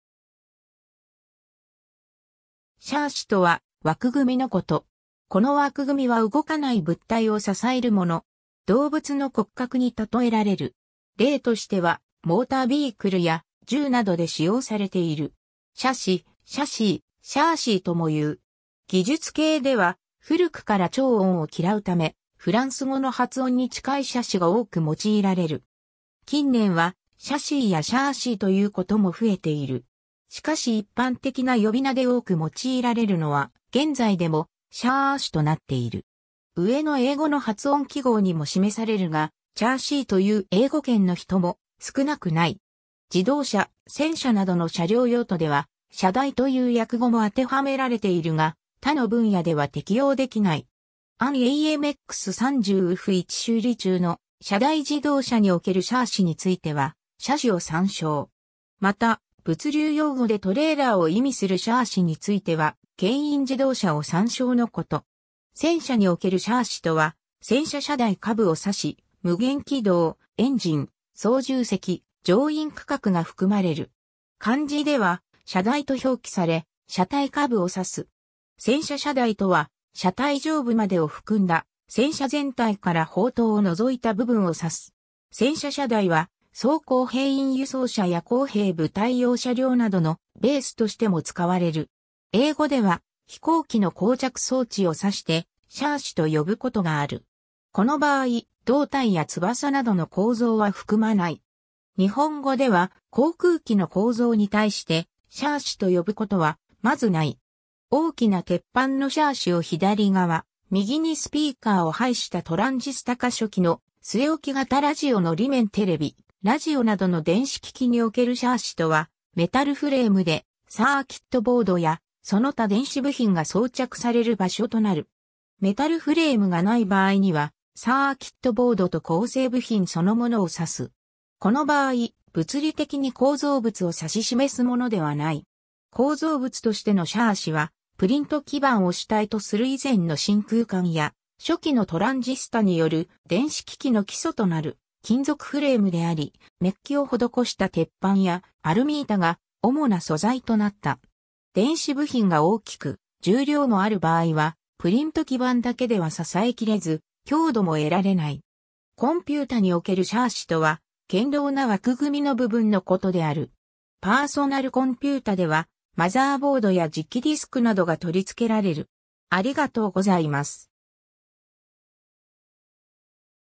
シャーシ
シャーシ（フランス語: châssis [ˈʃasi]、英語: chassis [ˈʃæsi(ː), ˈtʃæsi(ː)]）とは枠組み（フレームワーク）のこと。
この枠組みは動かない物体を支えるもの。
動物の骨格にたとえられる。
例としては、モーター・ビークルや銃などで使用されている。
 シャシ、シャシー、シャーシーともいう。
技術系では古くから長音を嫌うため、フランス語の発音に近いシャシが多く用いられる。
近年はシャシーやシャーシーということも増えている。
しかし一般的な呼び名で多く用いられるのは現在でもシャーシとなっている。
上の英語の発音記号にも示されるが「チャーシー」と言う英語圏の人も少なくない。
 自動車、戦車などの車両用途では車台という訳語も当てはめられているが、他の分野では適用できない。
 An AMX 30 AuF1 修理中の車台 自動車におけるシャーシについては、シャシ (自動車)を参照。
また物流用語でトレーラーを意味するシャーシについては牽引自動車を参照のこと。
 戦車におけるシャーシとは、「戦車車台」下部を指し、無限軌道・エンジン・操縦席・乗員区画が含まれる。
 漢字では「車台」と表記され、「車体」（hull：ハル）下部を指す。
 「戦車車台」とは、「車体」上部までを含んだ、戦車全体から砲塔を除いた部分を指す。
「戦車車台」は装甲兵員輸送車や工兵部隊用車両などのベースとしても使われる。
 英語では、飛行機の降着装置を指してシャーシと呼ぶことがある。
この場合、胴体や翼などの構造は含まない。
日本語では、航空機の構造に対してシャーシと呼ぶことはまずない。
 大きな鉄板のシャーシを左側、右にスピーカーを配したトランジスタ化初期の据え置き型ラジオの裏面 テレビ、ラジオなどの電子機器におけるシャーシとは、メタルフレームでサーキットボード（基板）やその他電子部品が装着される場所となる。
メタルフレームがない場合には、サーキットボードと構成部品そのものを指す。
この場合、物理的に構造物を指し示すものではない。
 構造物としてのシャーシは、プリント基板を主体とする以前の真空管や初期のトランジスタによる電子機器の基礎となる金属フレームであり、メッキを施した鉄板やアルミ板が主な素材となった。
電子部品が大きく重量もある場合はプリント基板だけでは支えきれず強度も得られない。
 コンピュータにおけるシャーシとは、堅牢な枠組みの部分のことである。
パーソナルコンピュータでは、マザーボードや磁気ディスクなどが取り付けられる。